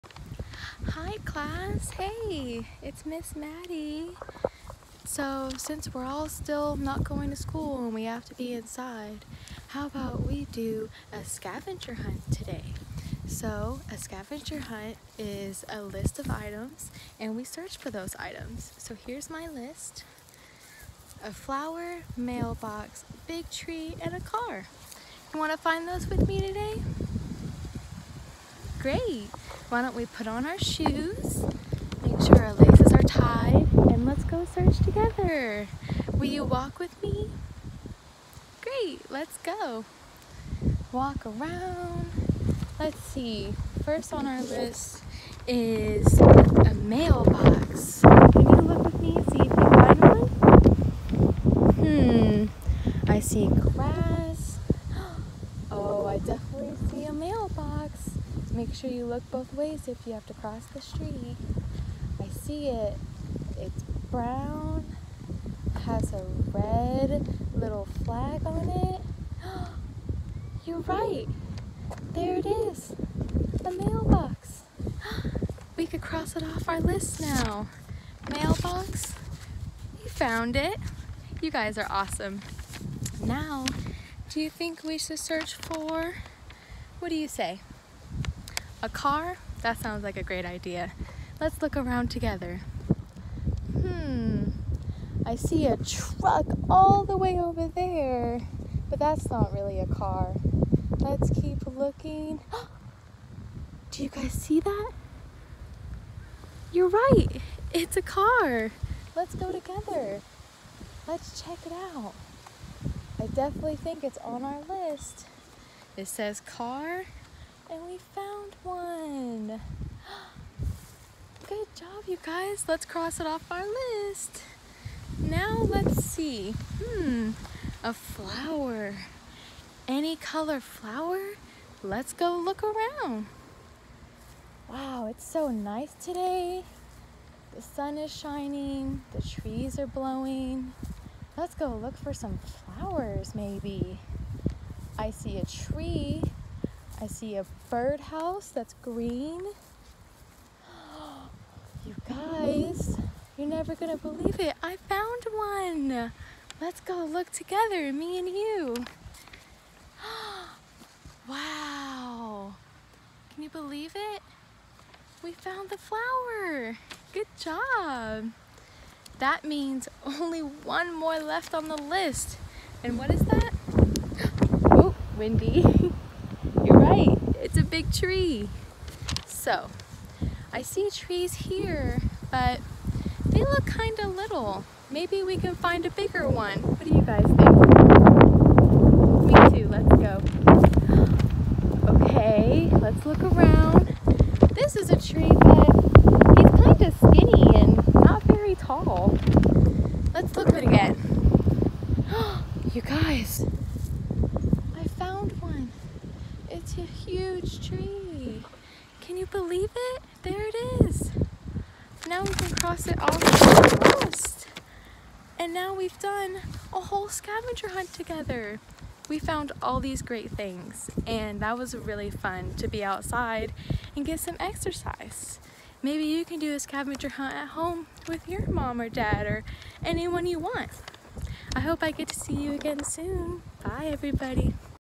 Scavenger Hunt !
Get outside with me and let’s find the items on our scavenger hunt list